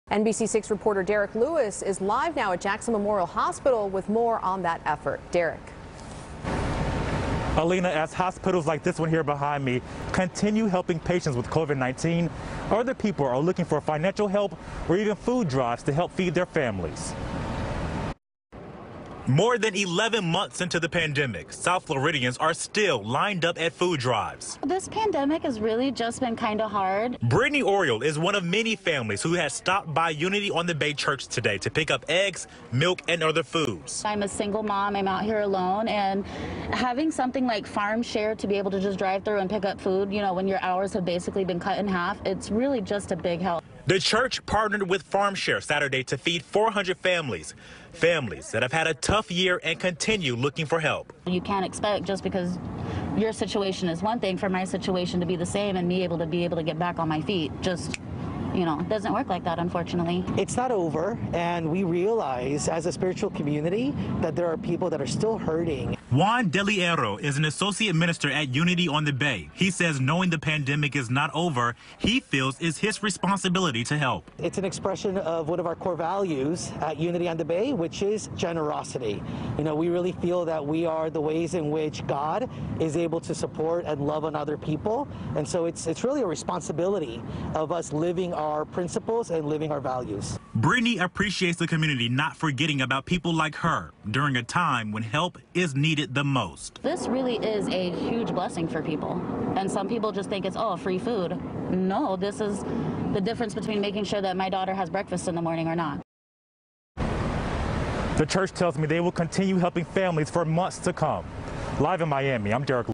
We gave away 400 meals to people in need
NBC6 caught us in the act of giving away food on Saturday. This was our third food distribution and we have more to come.